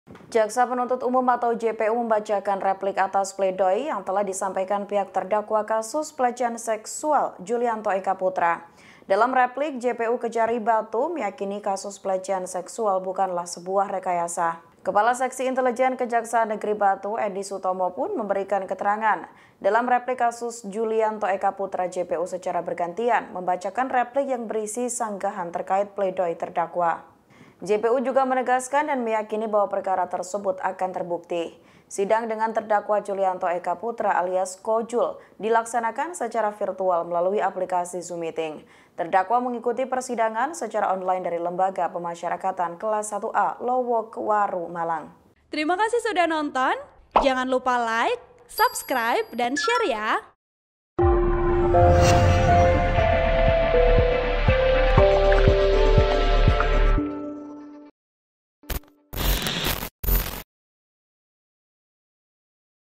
Bacakan Replik atas Pledoi, JPU Kejari Batu Sebut Pelecehan Julianto Eka Putra Bukan Rekayasa
TRIBUN-VIDEO.COM - Jakas Penuntut Umum (JPU) membacakan replik atas pledoi yang telah disampaikan pihak terdakwa kasus pelecehan seksual, Julianto Eka Putra.

Dalam replik, JPU Kejari batu meyakini kasus pelecehan seksual bukanlah sebuah rekayasa.

Kepala Seksi Intelijen Kejaksaan Negeri Batu, Edi Sutomo pun memberikan keterangan.

Host: Mufti Fauziah
VP: Indra Imawan